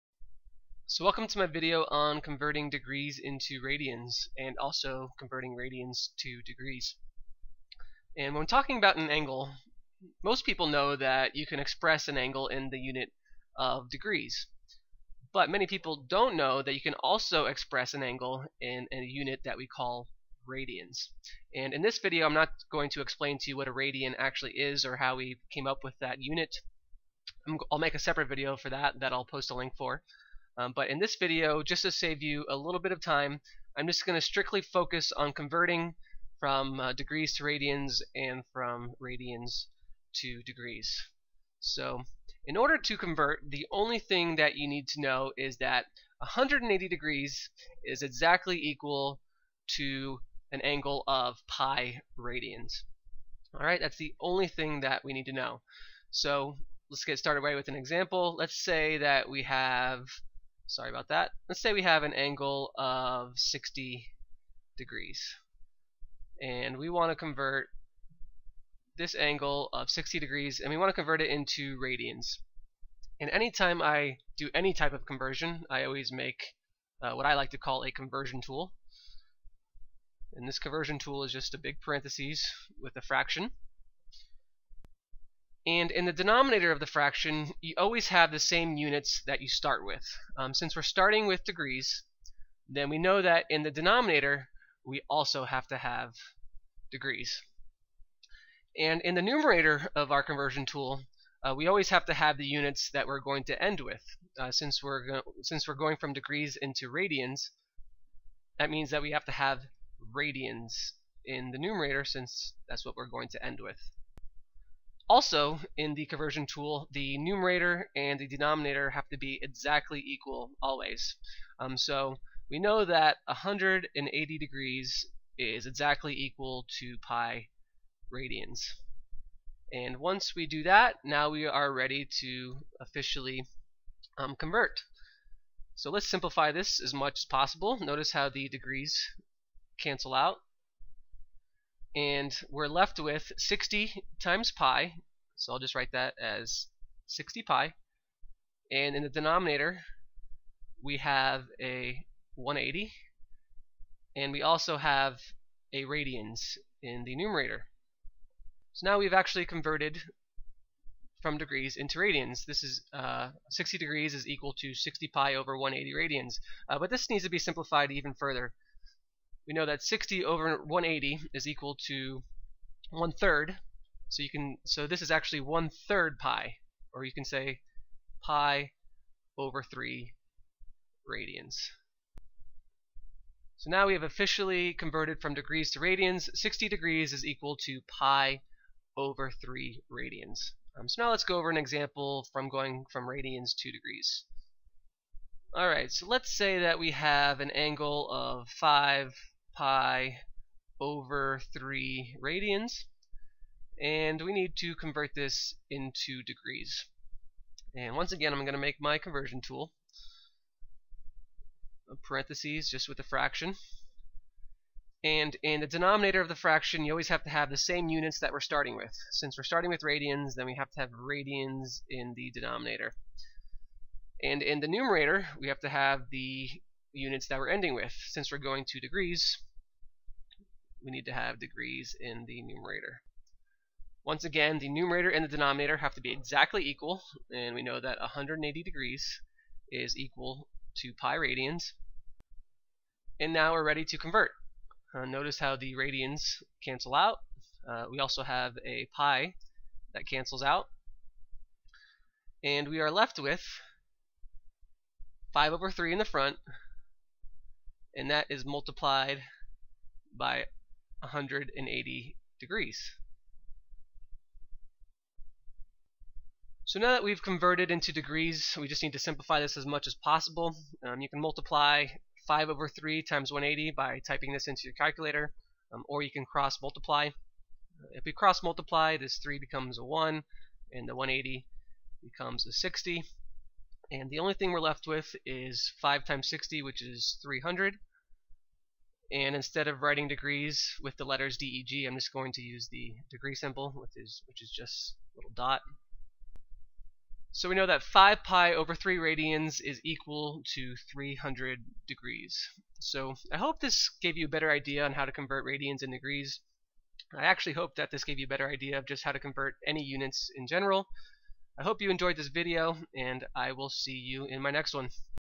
Converting radians and degrees
Learn how to convert radians to degrees in this step by step example.